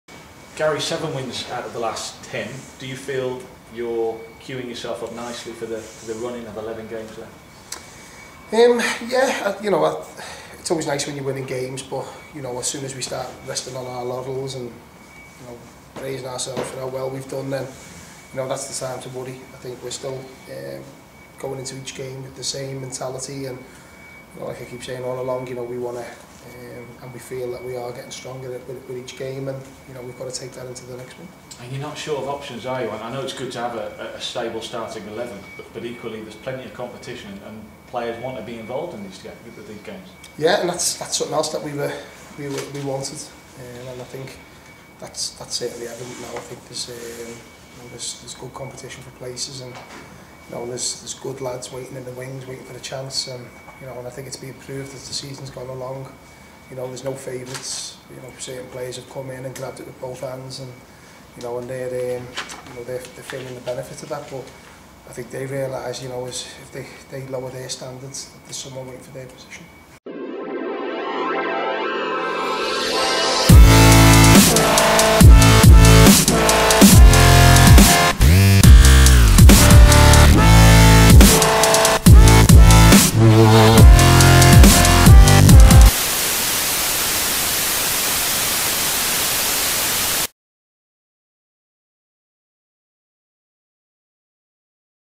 Pre-Match Interview: Gary Brabin (Gateshead home)
Tranmere manager Gary Brabin looks ahead to Saturday's home match against Gateshead.